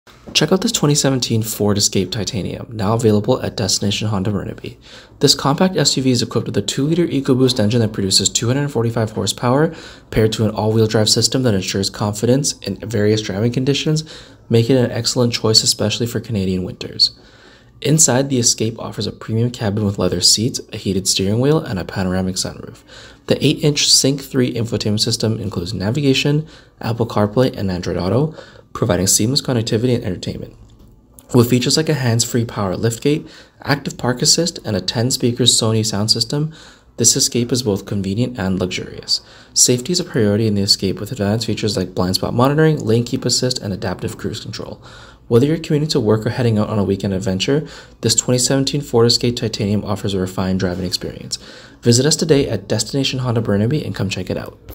2017 FORD ESCAPE TITANIUM - FULLY LOADED - VERSATILE SUV! - B13262
The 2017 Ford Escape Titanium is a compact SUV that offers a perfect mix of style, performance, and advanced technology. Equipped with a 2.0-liter EcoBoost engine that produces 245 horsepower, this Escape delivers lively acceleration and a responsive driving experience. The all-wheel-drive system ensures confidence in various driving conditions, making it an excellent choice for Canadian winters.

Inside, the Escape Titanium offers a premium cabin with leather-trimmed seats, a heated steering wheel, and a panoramic sunroof. The 8-inch touchscreen SYNC 3 infotainment system includes navigation, Apple CarPlay, and Android Auto, providing seamless connectivity and entertainment. With features like a hands-free power liftgate, active park assist, and a 10-speaker Sony sound system, this Escape is both convenient and luxurious.

Safety is a priority in the Escape Titanium, with advanced features like blind-spot monitoring, lane-keeping assist, and adaptive cruise control. Whether you’re commuting to work or heading out on a weekend adventure, the 2017 Ford Escape Titanium offers a refined driving experience.

VSA#30902